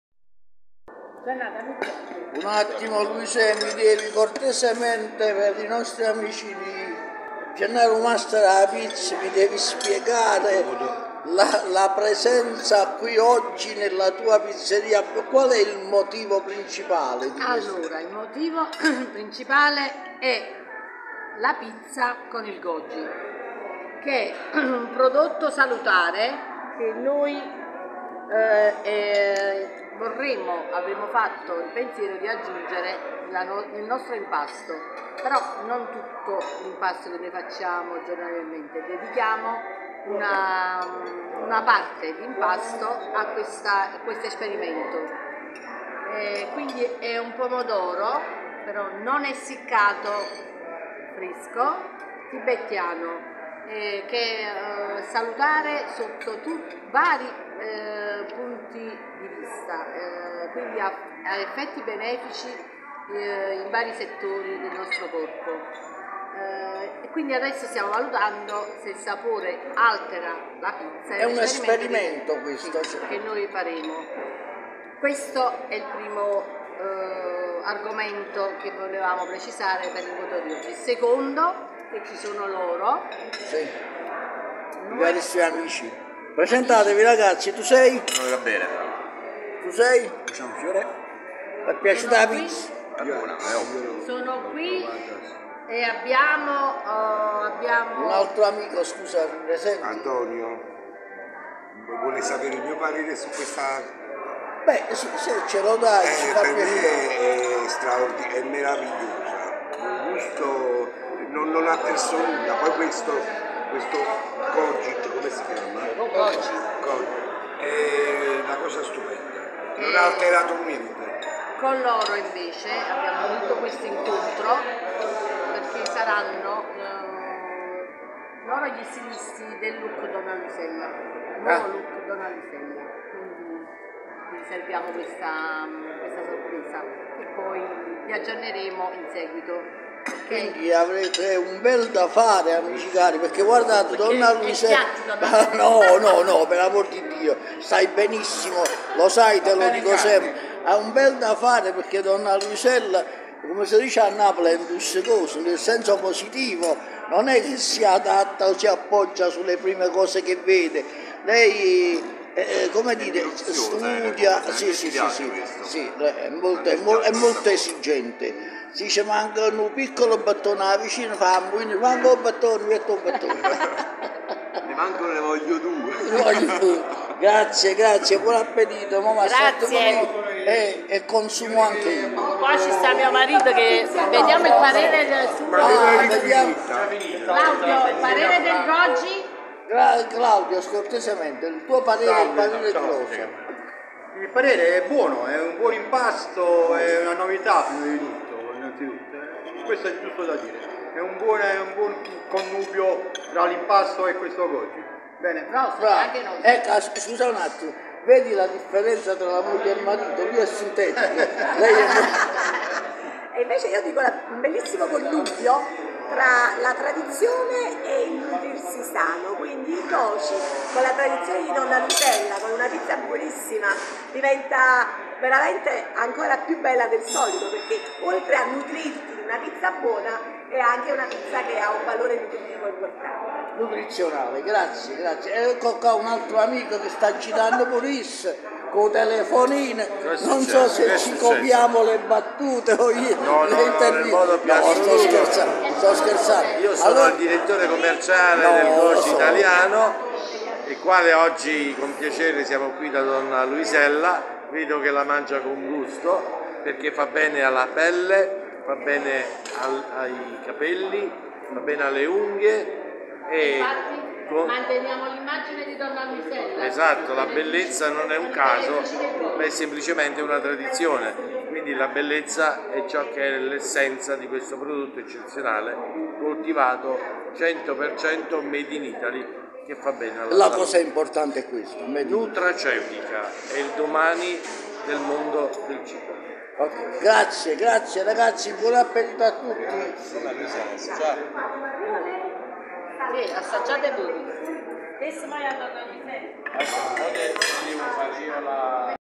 Donna Luisella Benvenuti al Sud
Prove e degustazione di pizze al Gòji italiano